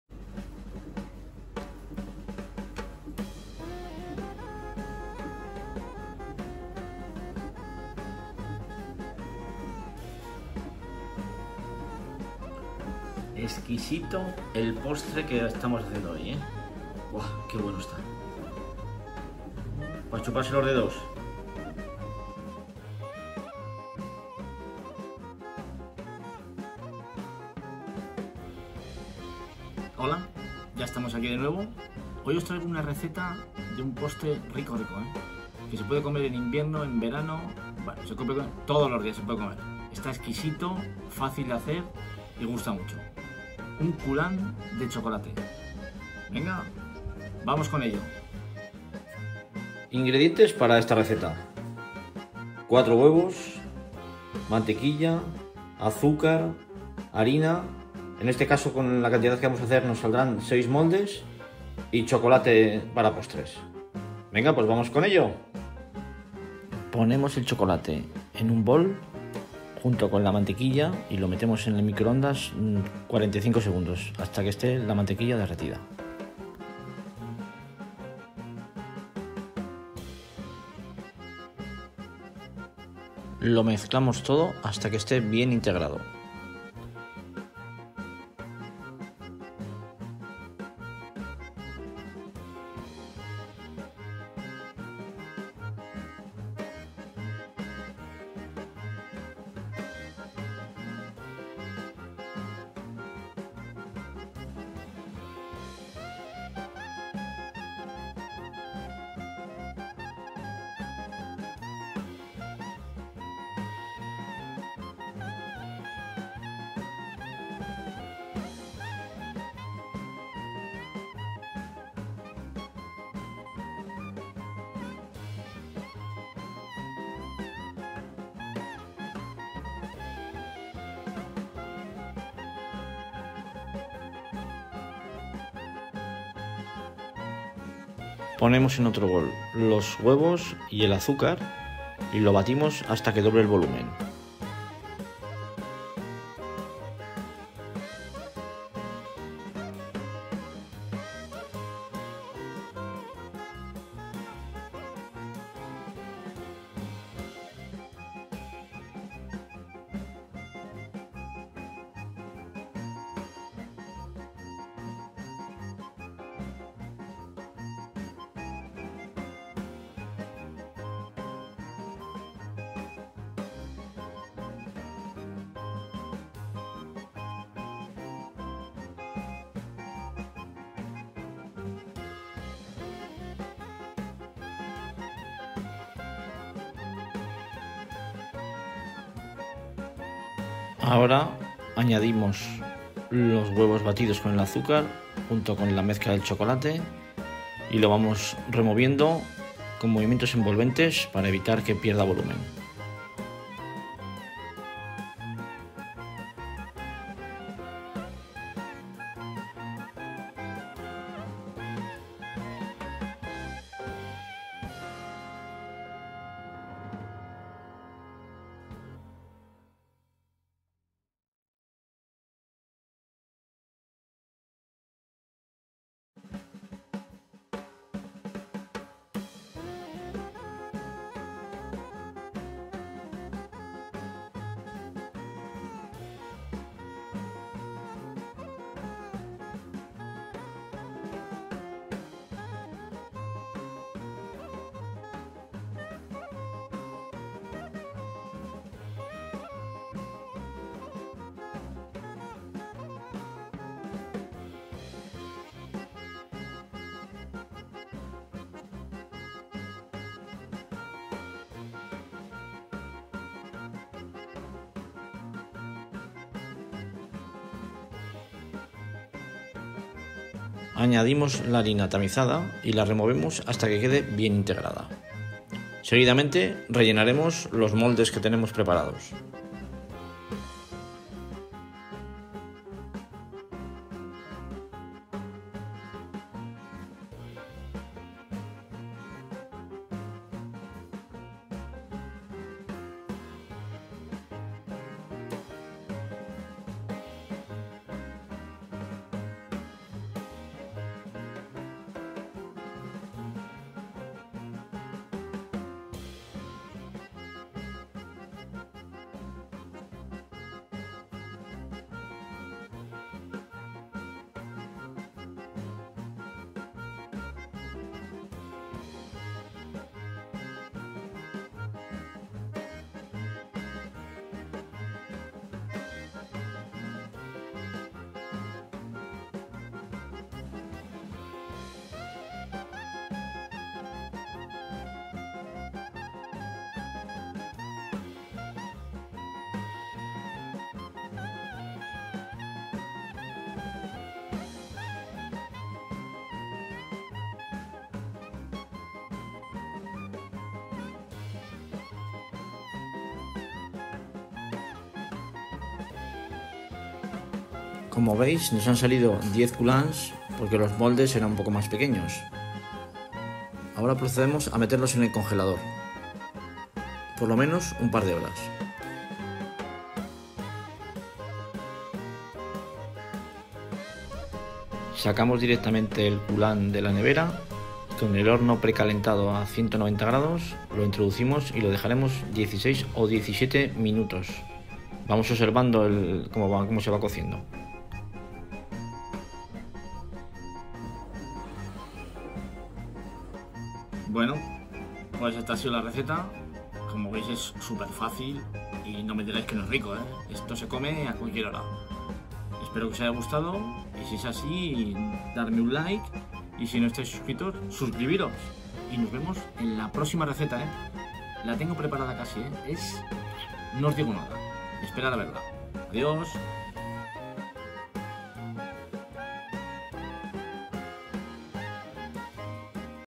RECETA DELICIOSA para AMANTES del CHOCOLATE. COULANT DE CHOCOLATE.
Ingredientes;
-150 gr de chocolate para postres
-150 gr de mantequilla
-4 huevos
-100 gr de azúcar
-70 gr de harina
- Helado de vainilla para acompañar

Elaboración:
poner en un bol el chocolate troceado y la mantequilla, lo introducimos en el microondas durante 45 segundos, no hay que tenerlo demasiado tiempo, para evitar quemar el chocolate, sacamos el bol y lo removemos hasta que quede bien integrado. En otro bol batimos los huevos junto con el azúcar , hasta que doble el volumen.
Ahora mezclamos los huevos con el azúcar y el chocolate con mantequilla, se mezcla hasta que quede bien integrado. Por un ultimo tamizamos la harina y se la añadimos a la mezcla, con movimientos envolventes , para evitar que pierda volumen.
Rellenamos los moldes y los llevaremos al congelador por lo menos 2 horas.
Cuando vayamos a preparar el postre, precalentamos el horno a 190 º con calor arriba y abajo, cuando este caliente introducimos nuestro postre, el tiempo de horneado es fundamental, depende de cada horno, suele ser 16 o 17 minutos, pero lo mejor en meter de uno para probar el tiempo.
Y ya lo tenemos hecho , como veis es un postre fácil, que se puede tener en el congelador y en un cuarto de hora podemos disfrutar de él, si le ponéis un poco de helado de vainilla, ya es lo mas.
Os espero en la próxima.
Espero vuestros comentarios , sugerencias y sobre disfrutad de la cocina.